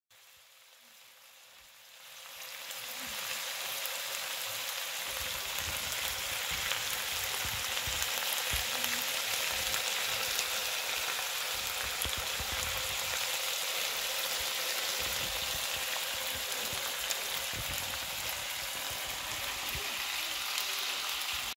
주철 그리들 닭갈비 요리
캠핑전 집에서 사용해 봤습니다~ 자세한 시즈닝 방법과 요리영상은 조금만 기다려 주세요~^^